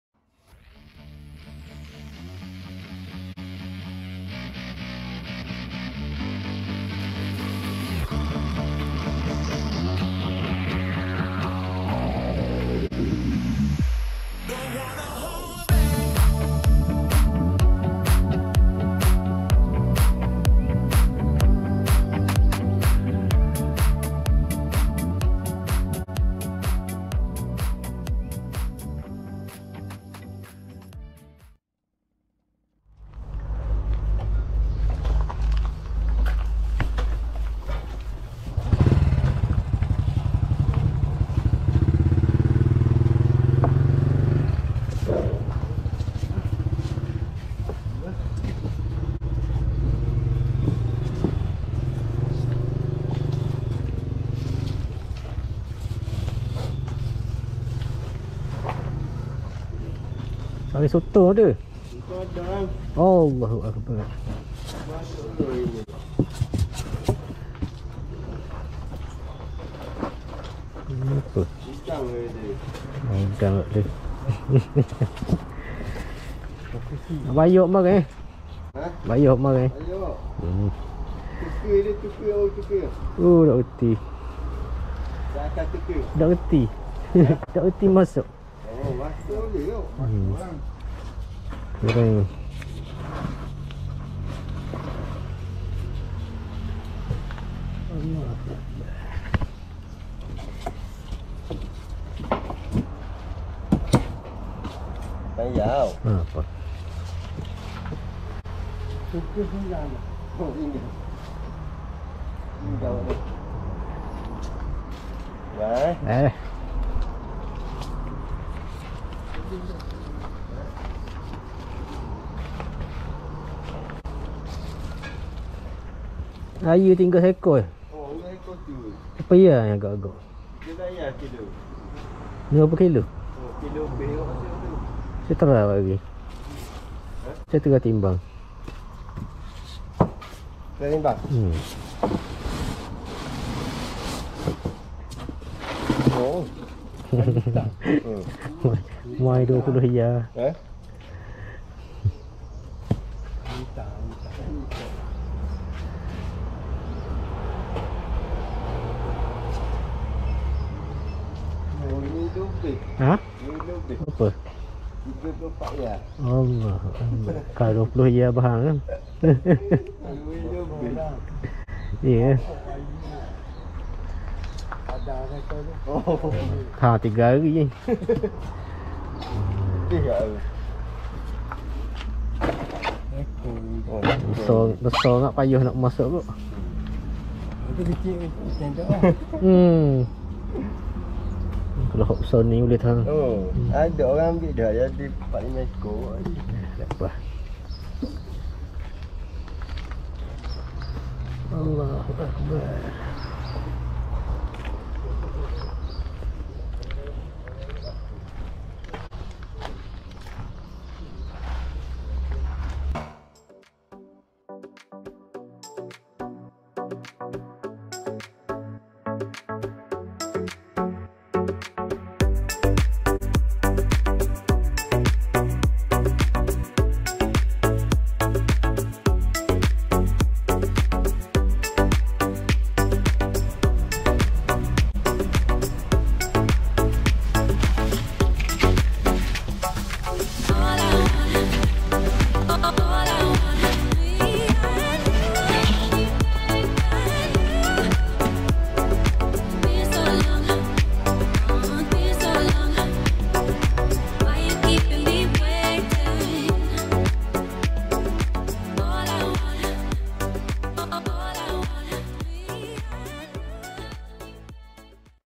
Misi Bayar Hutang Berjaya | Tapi Misi Cari Sotong Gagal | Gerai Barang Basah Seberang Marang
rakaman pada 7 julai 2022. edit pada 11 julai 2022.